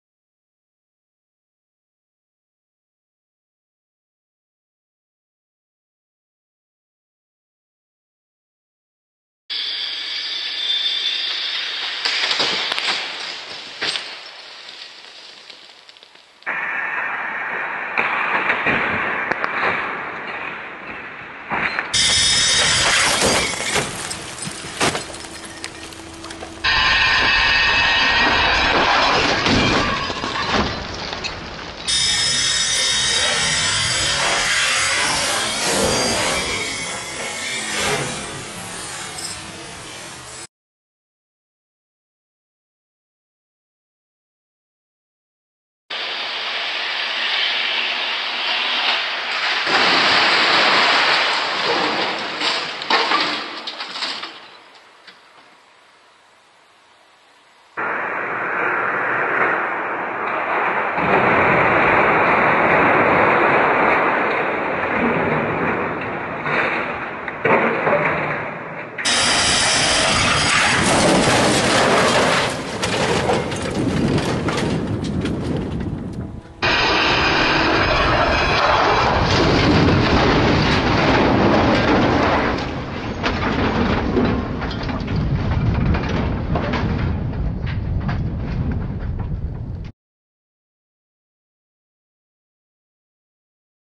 Crash barrier test
Precast concrete crash barrier reinforced with Basalt Fibre Reinforced Polymer (BFRP) sand coated bars. BFRP is 2.5 stronger and 6 x lighter than steel it is unaffected by road salts and therefore does nor suffer corrosion from chlorides or carbonation. Makes for lighter barriers - more barriers per load.